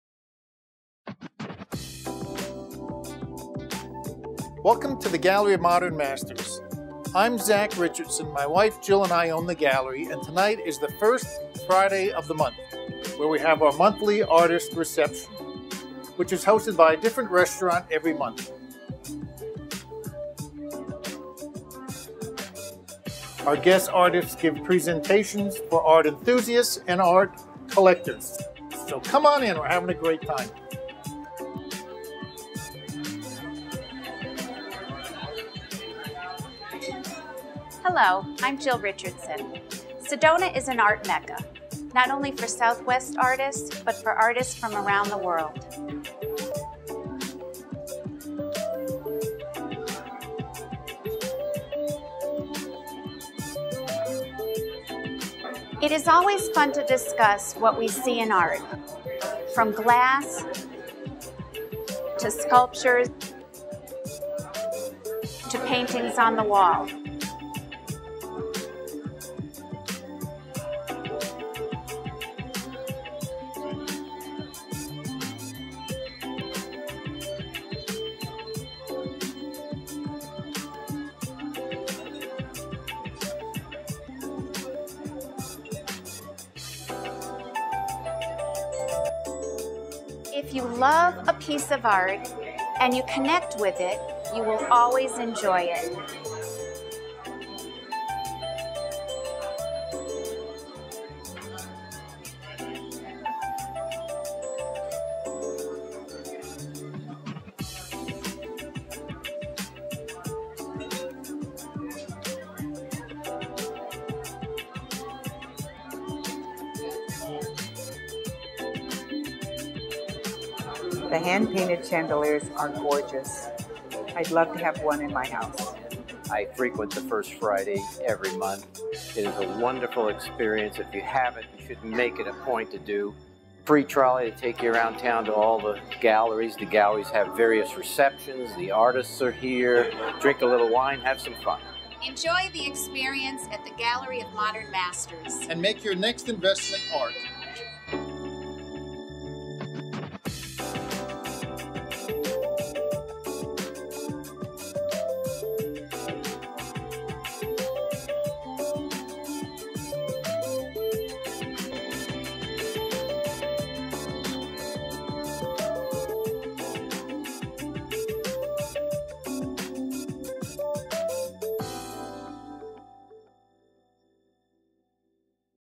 Gallery Of Modern Masters Celebrates First Friday at Hillside Sedona
The Gallery of Modern Masters in Hillside Sedona participates each month in the Sedona Gallery Association's First Friday Art Walk. Enjoy this video of Gallery of Modern Masters owners Jill and Zach Richardson talking a bit about the First Friday event and of art lovers having a great time.